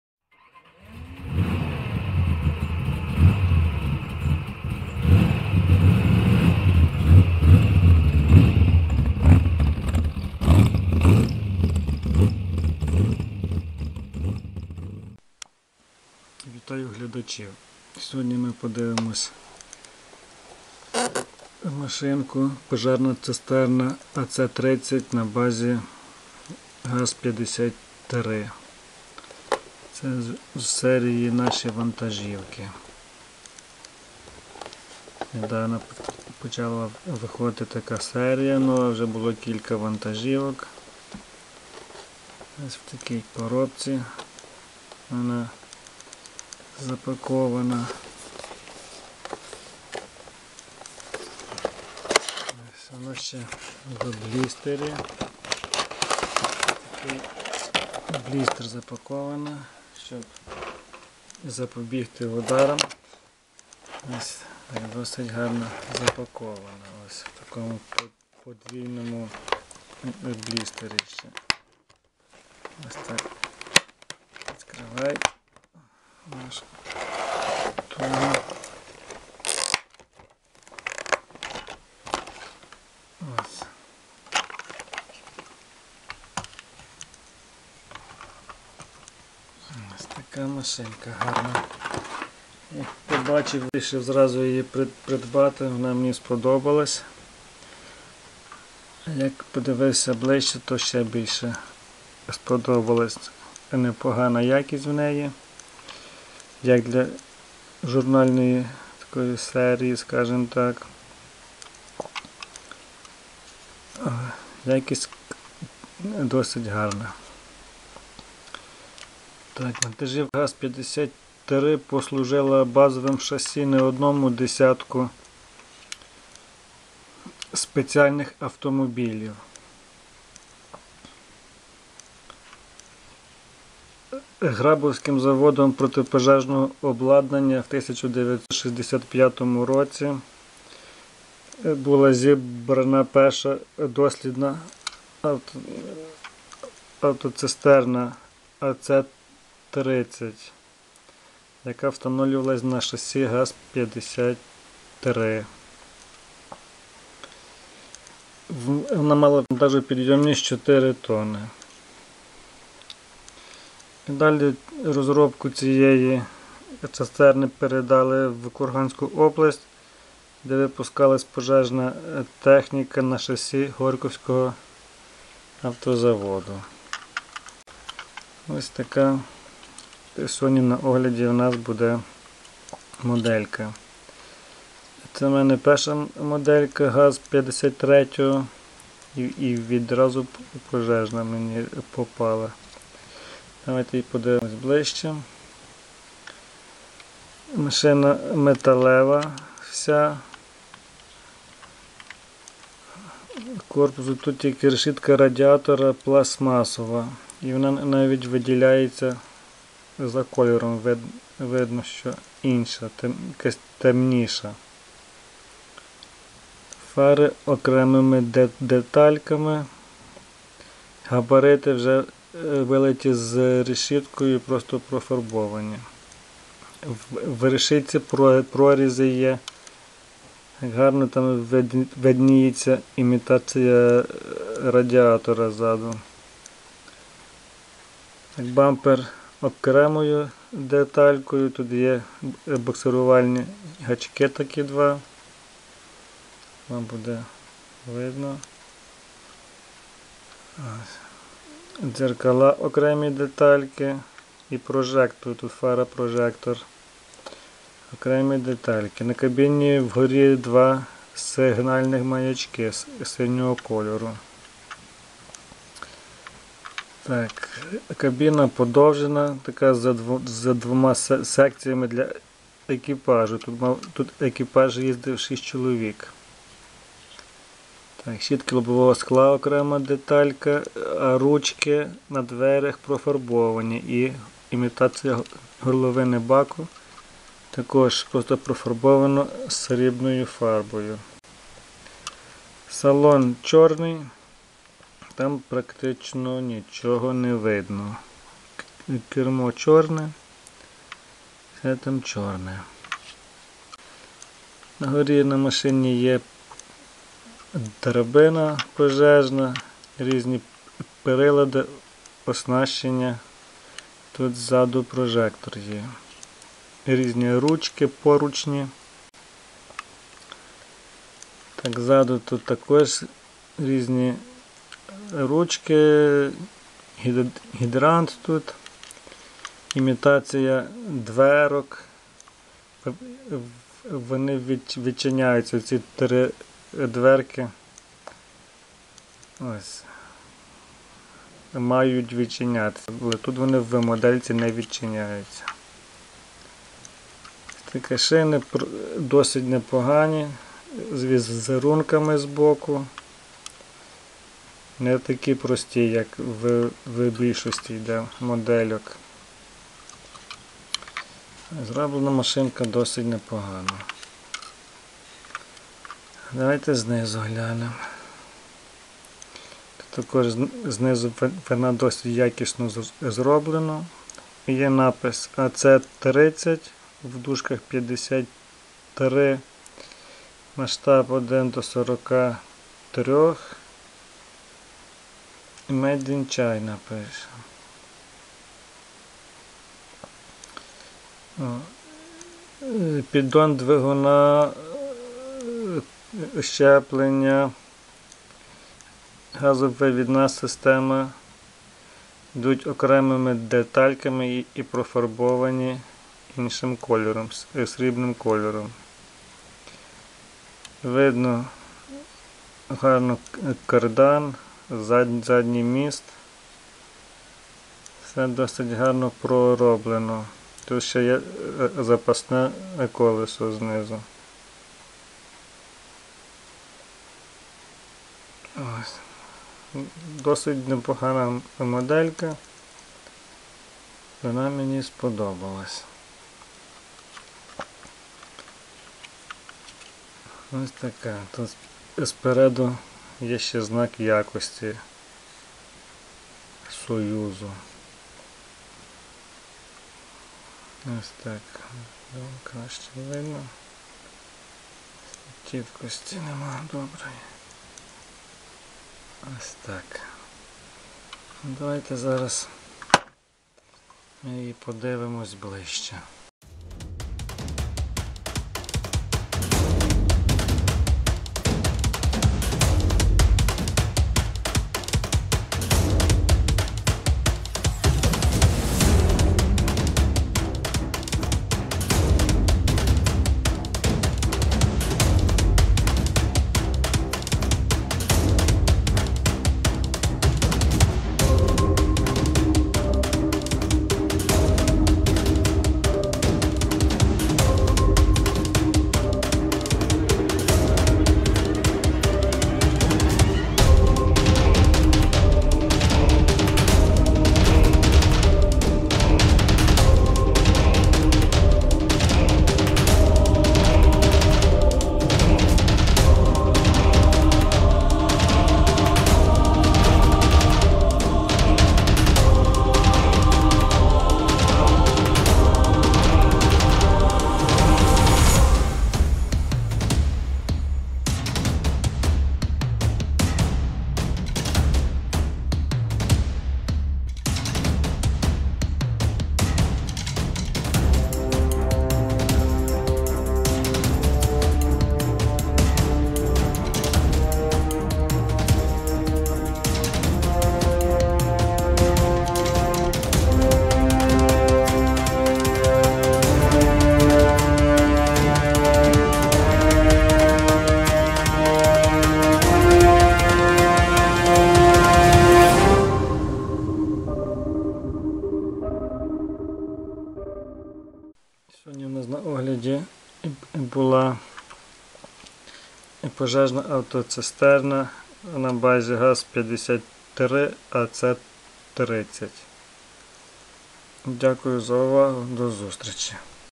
Пожежна автоцистерна АЦ-30(53),Наші Вантажівки.
Огляд пожежної цистерни АЦ-30 на базі ГАЗ-53,з серії Наші Вантажівки.